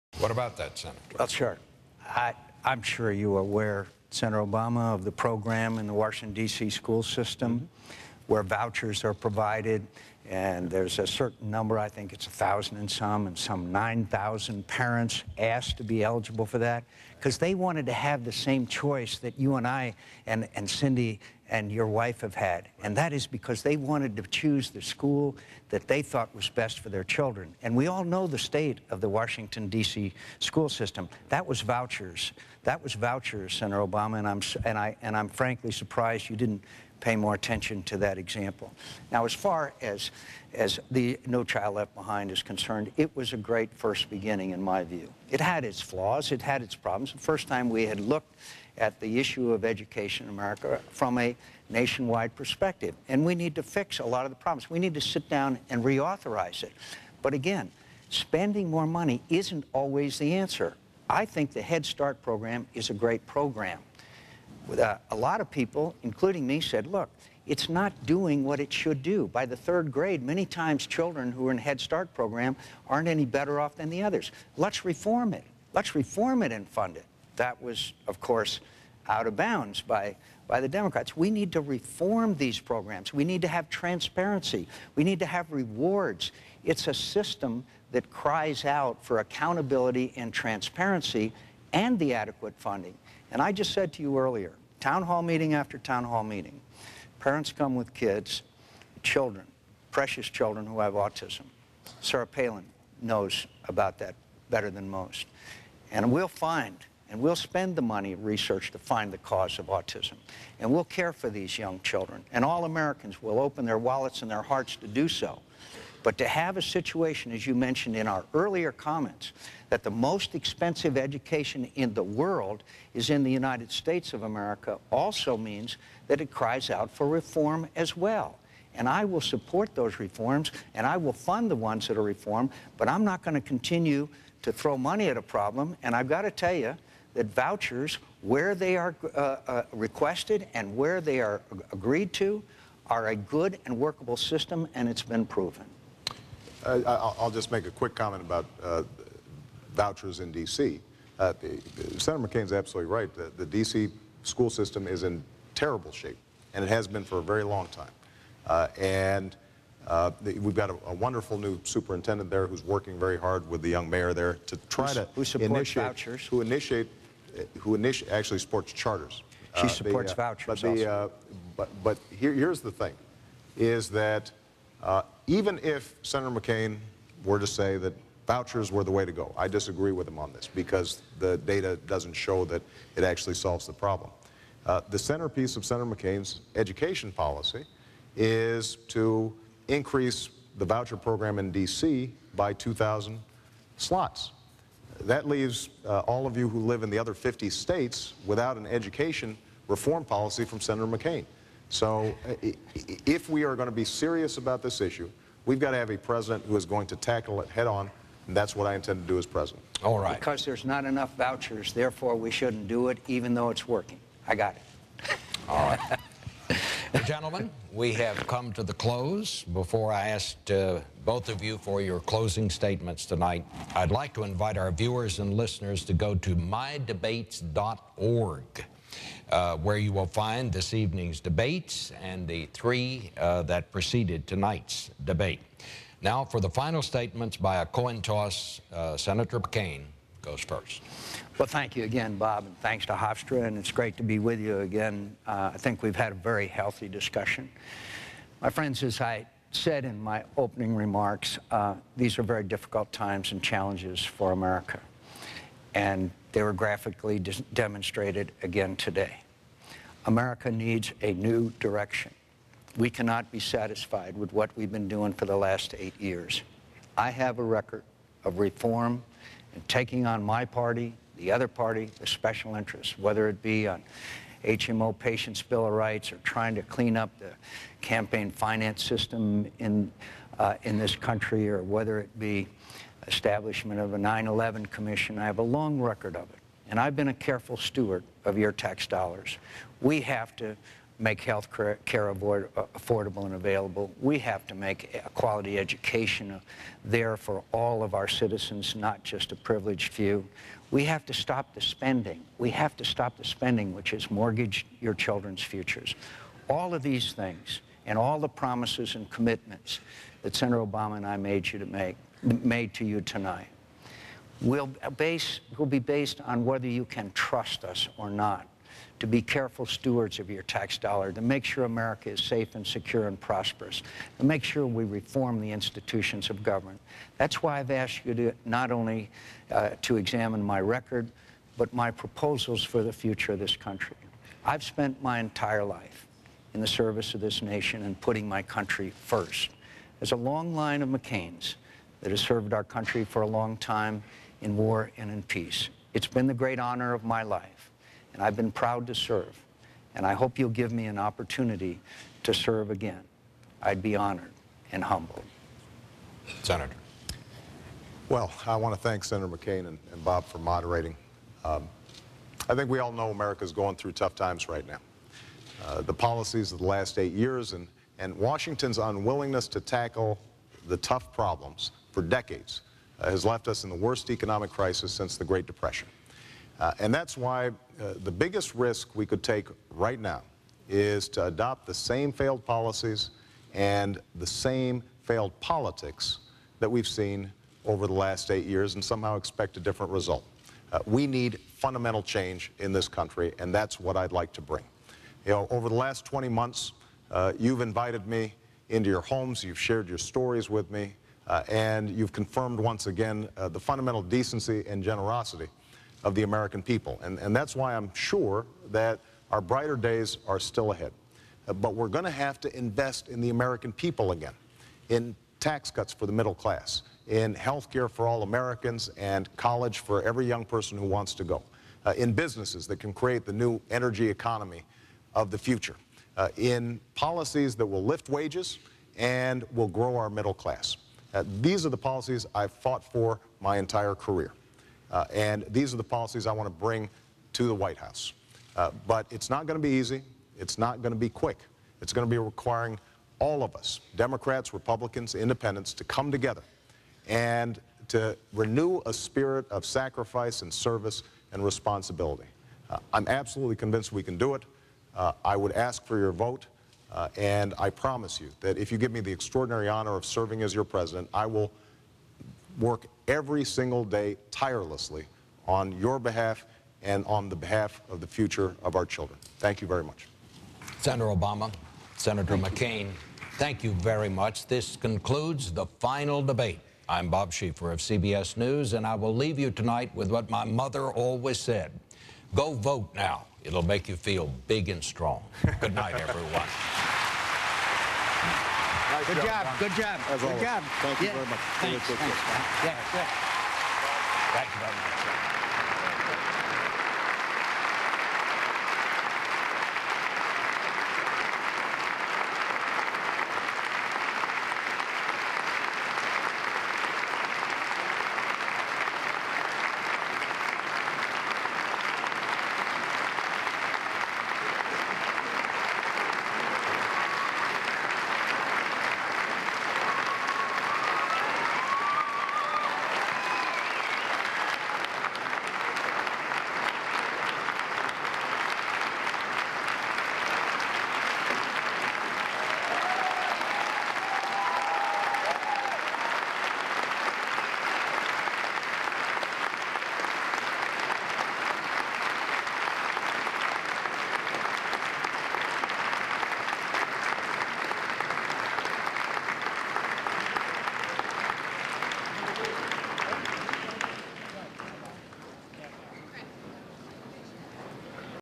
Part 10 of 10 - Third and Final Presidential Debate - John McCain and Barack Obama, October 15, 2008
Part 10 of 10 of the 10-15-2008 Presidential Debate between Senator Barack Obama and Senator John McCain.